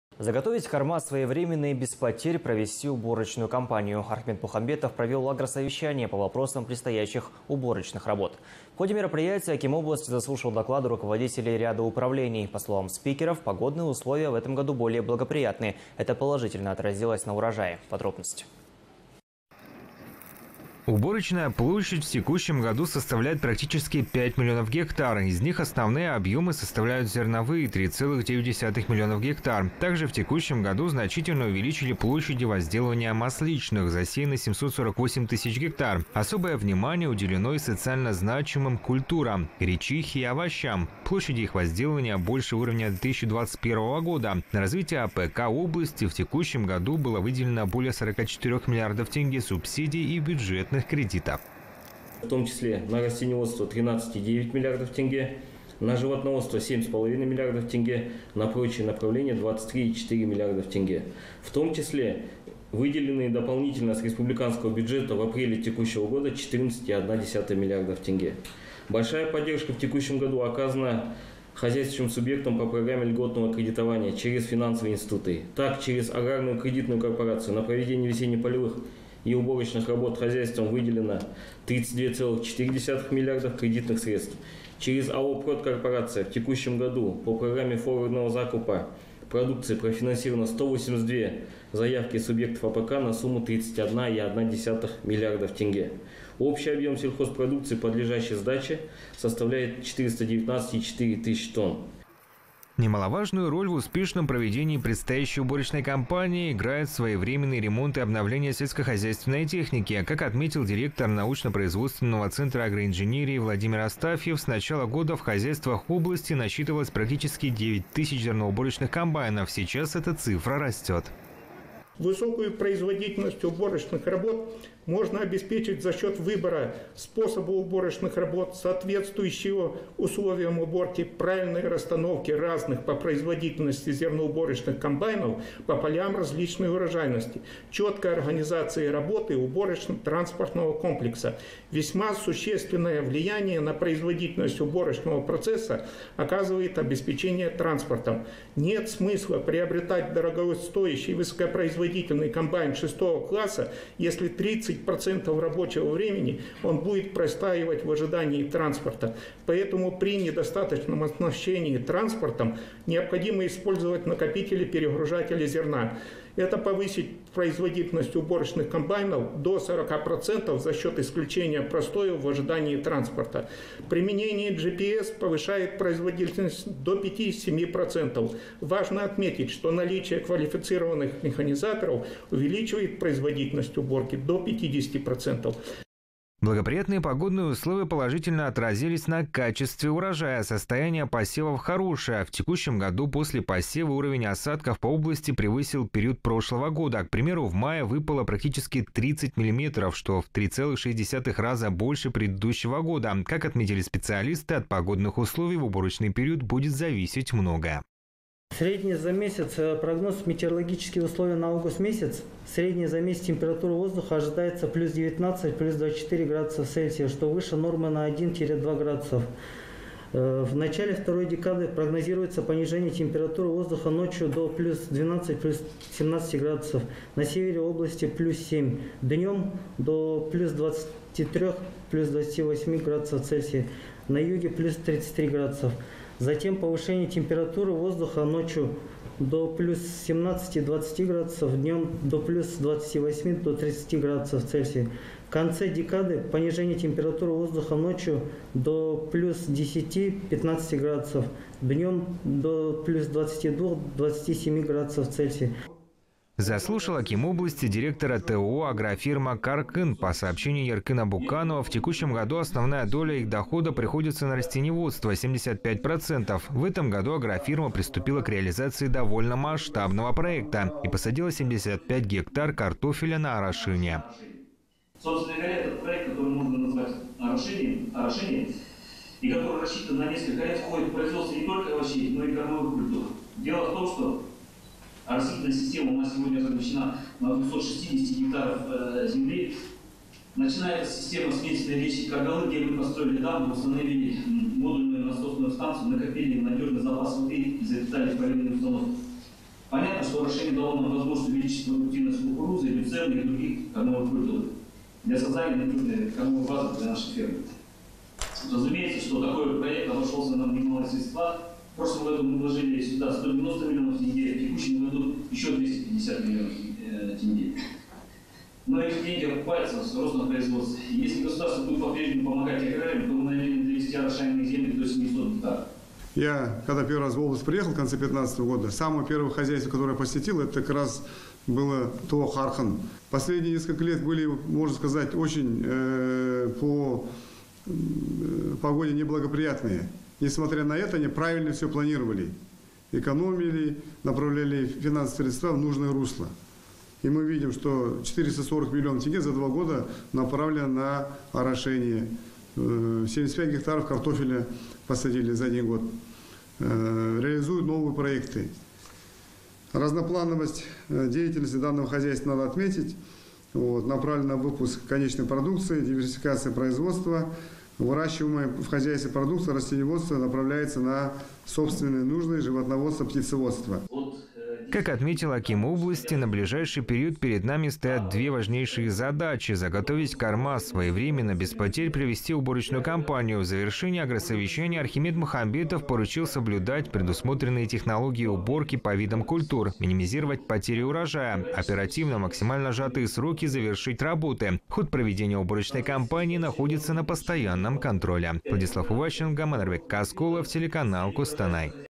Уборочная кампания не за горами
Заготовить корма своевременно и без потерь провести уборочную кампанию. Архимед Мухамбетов  провел агросовещание по вопросам предстоящих уборочных работ.  В ходе мероприятия аким области заслушал доклады  руководителей ряда управлений.  По словам спикеров, погодные условия в этом году более благоприятны, это положительно отразилось на урожае.

Уборочная площадь в текущем году составляет практически 5 млн. га, из них основные объемы составляют зерновые 3,9 млн. га. Также, в текущем году значительно увеличили площади возделывания масличных - засеяно 748 тыс. га. Особое внимание уделено и социально значимым культурам, гречихе и овощам, площади их возделывания больше уровня 2021 года. На развитие АПК области в текущем году было выделено более 44 млрд. тенге. субсидий и бюджетных кредитов.